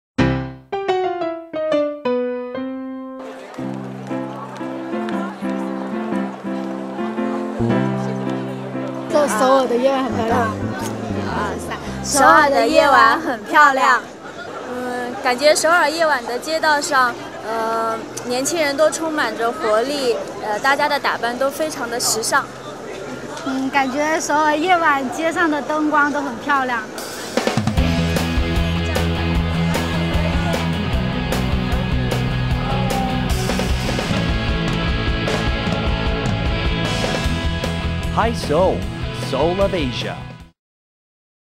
首尔是_夜晚很漂亮
首尔的夜晚很漂亮。
感觉首尔夜晚的街道上年轻人都充满着活力，大家的打扮都非常的时尚。
感觉首尔夜晚街上的灯光都很漂亮